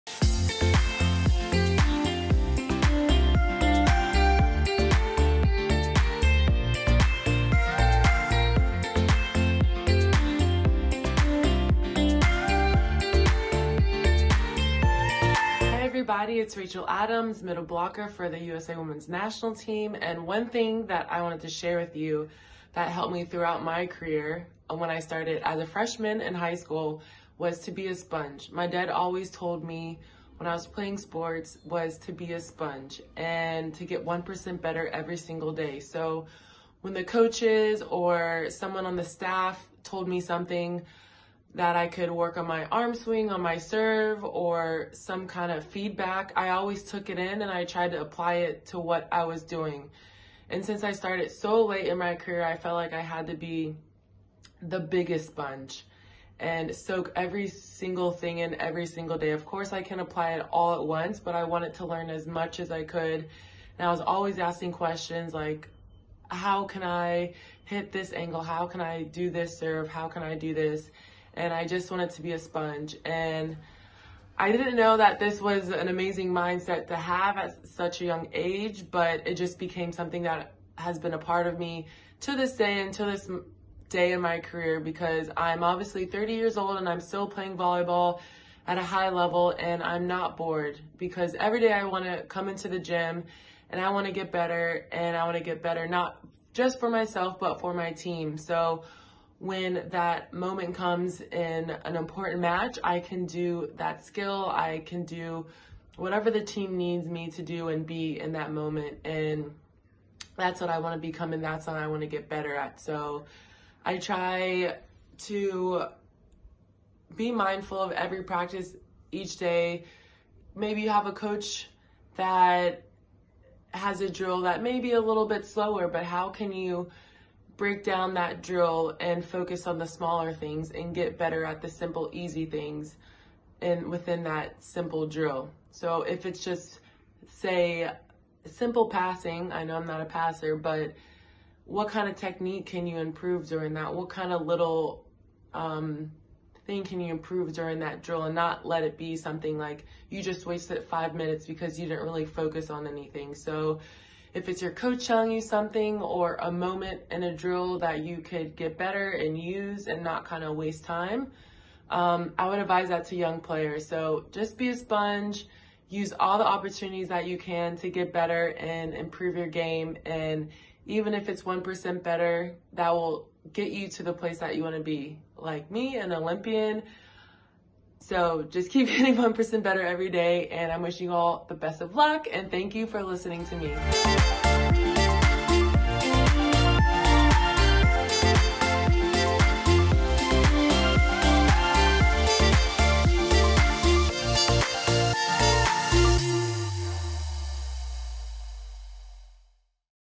Rachael Adams | Be a Sponge | USAVlearn
U.S. Women's National Team athlete Rachael Adams heard "Be a Sponge" all the time in high school. Being a sponge - listening to critiques and welcoming lessons and observations from others - was critical for her development as a player.

Look and feel like a pro in the latest USA Volleyball gear!